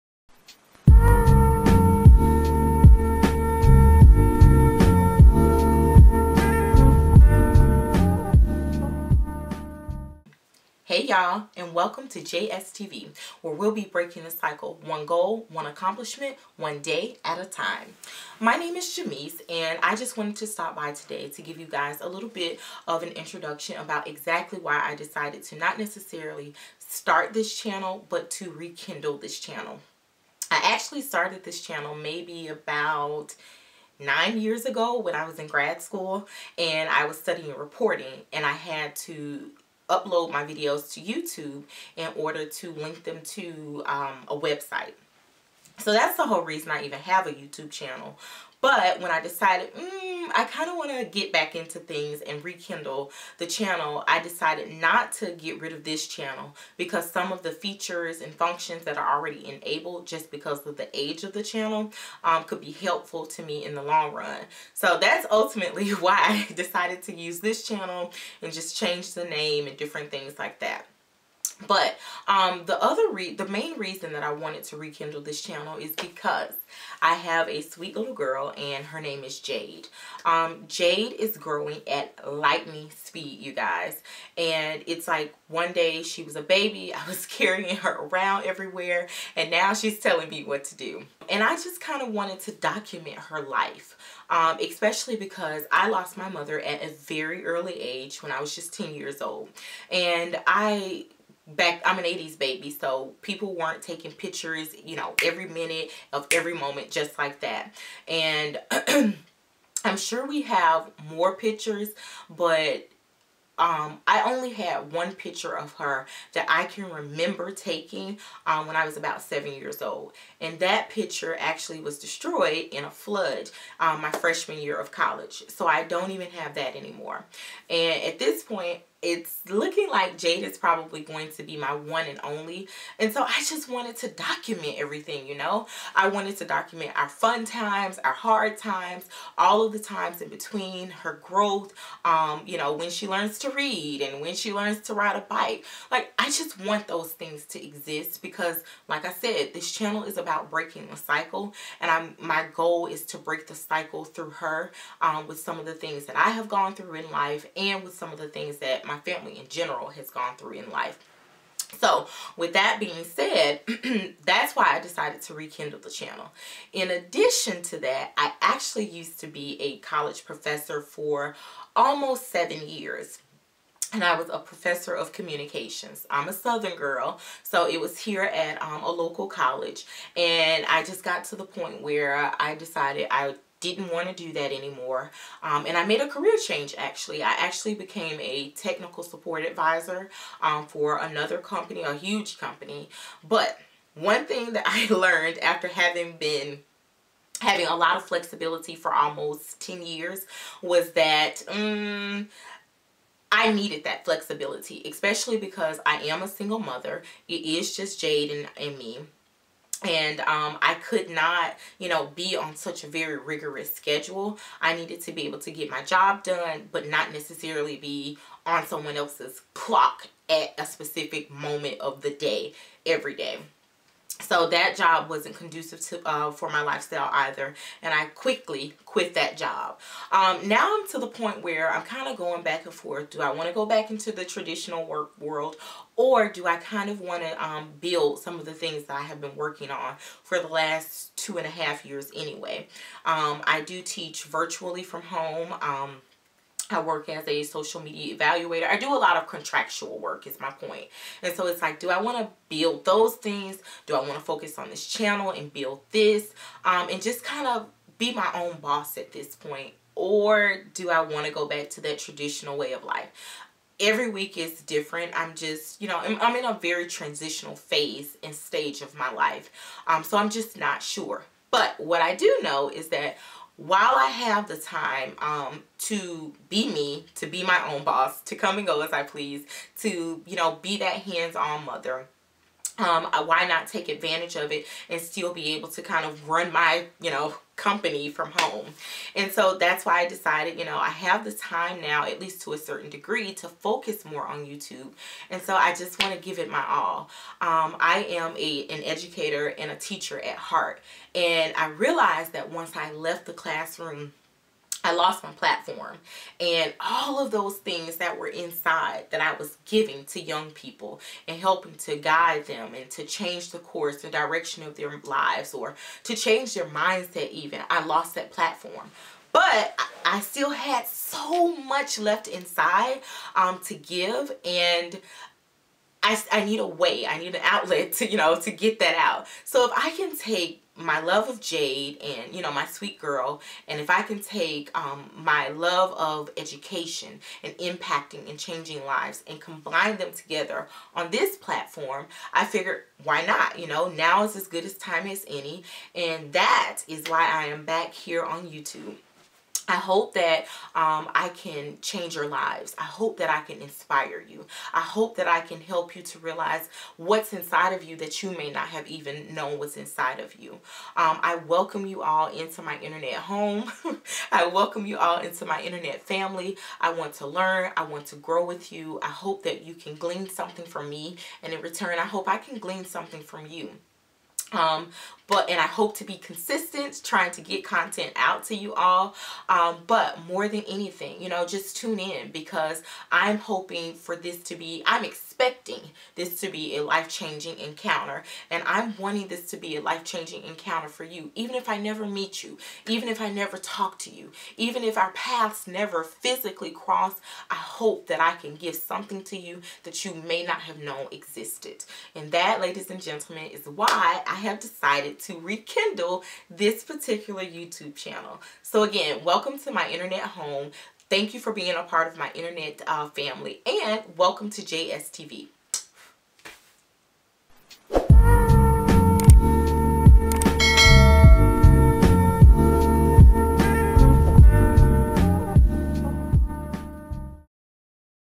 Welcome to Jae S. TV
Hey, hey my new internet family!  This is just a brief intro video to Jae S. TV.  I'm Jamese and I hope you enjoy my very first video as a re-kindled and revamped channel.